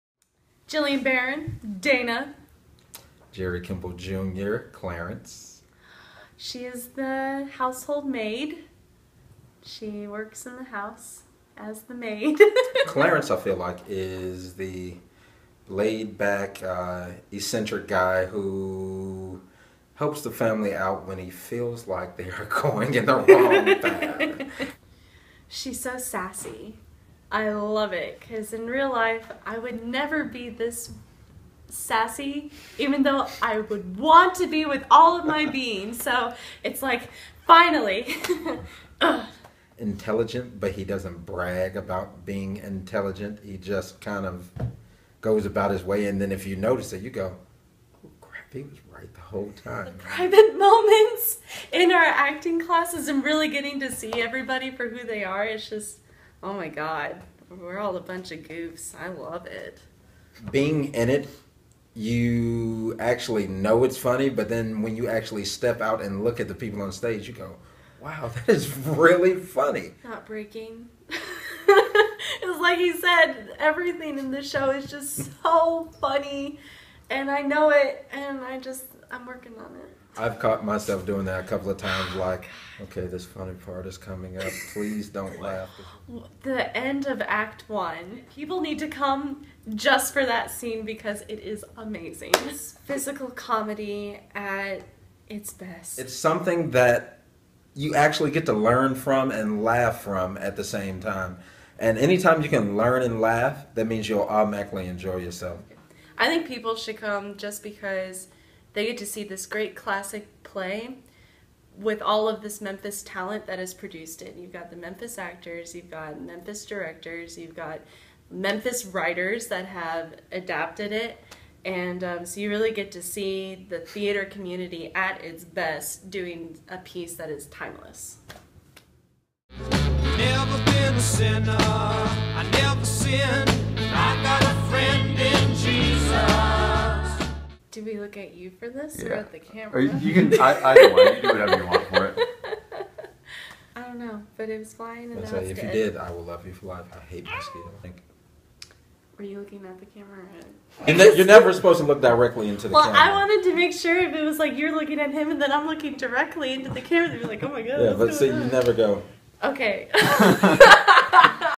blessed Jillian and Jerry
Cast members Jillian Barron and Jerry Kimble, Jr. talk about 3PT's original new work - blessed. Also outtakes.

blessed is an original transdaptation of Tartuffe, by Moliere - updated and set in Memphis.

"This laugh-out-loud comedy hits home, right below the Bible-belt."